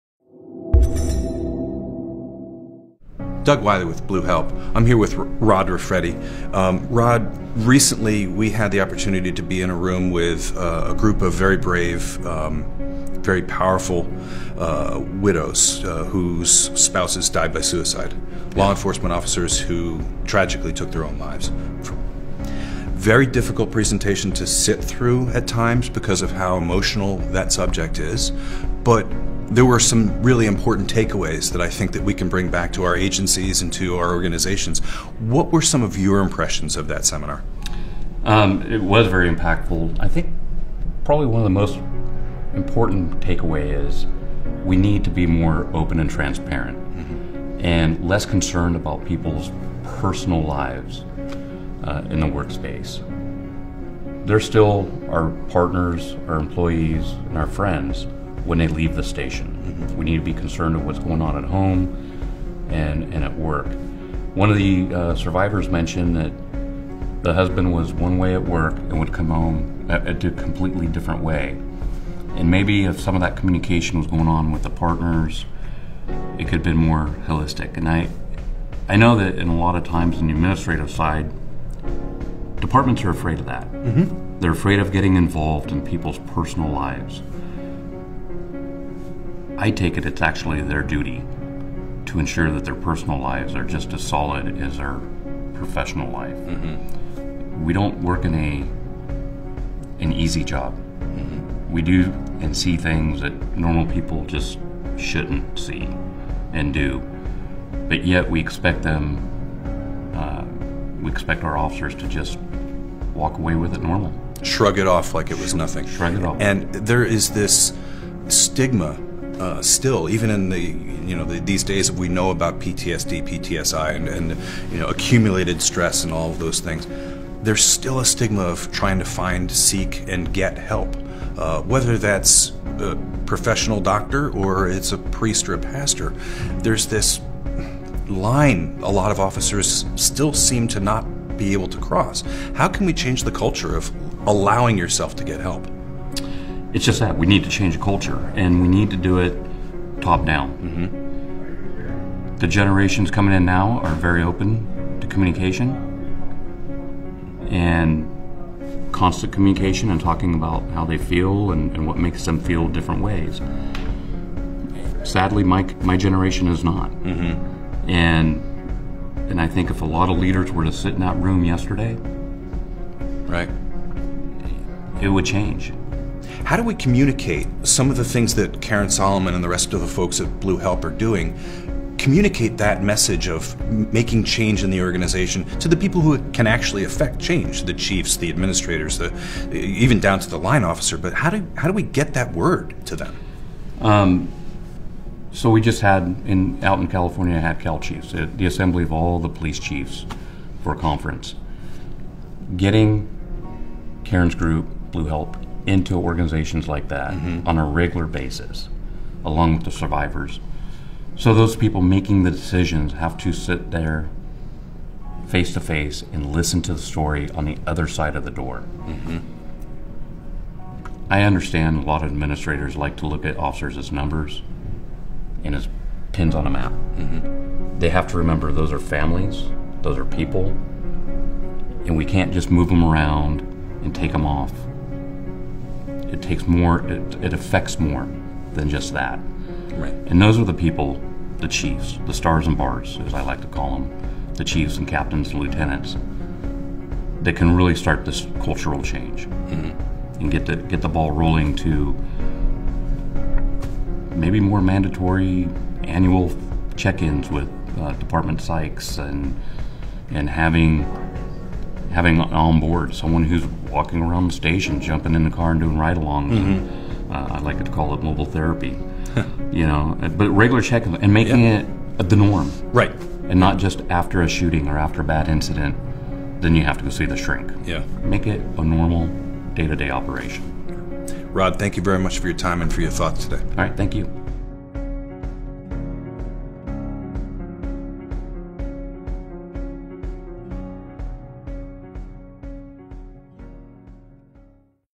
Culture Change in Law Enforcement
Another great piece from our friends at Virtual Academy. Rod Rifredi was kind enough to discuss his thoughts on culture change and suicide.

"I understand a lot of administrators like to look at officers as numbers and as pins on a map. They have to remember those are families, those are people, and we can’t just move them around and take them off. It affects more than just that. And those are the people, the stars and bars, that can really start the cultural change."